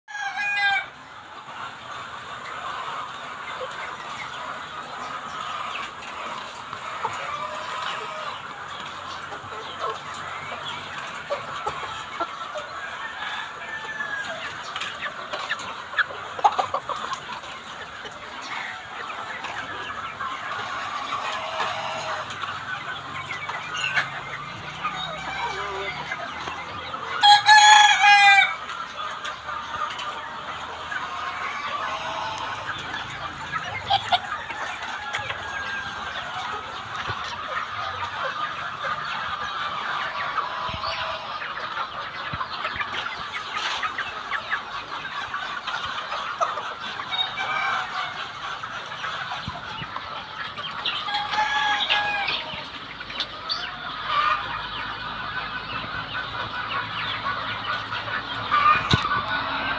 Kandang Galvanis Baterai Ternak Ayam Kampung Pedaging solusi lahan minim perkotaan
Solusi untuk beternak di kota yang sulit lahan luas yaitu dengan menggunakan kandang baterai galvanis sehingga kontrol bobot ayam dapat dipantau dengan jelas.